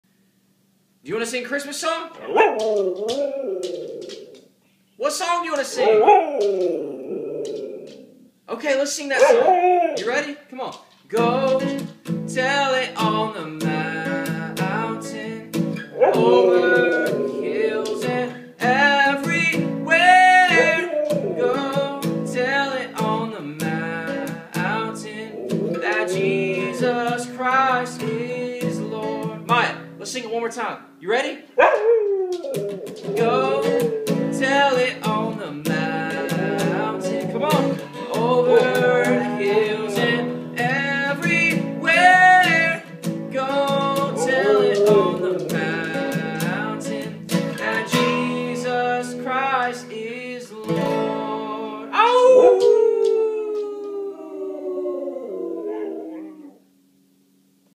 Siberian Husky Helping Me Sing Christmas Carol - Sort of.
Random video we shot with our dog Maya.  She likes to howl so we thought we'd see how far we could take it.  Enjoy.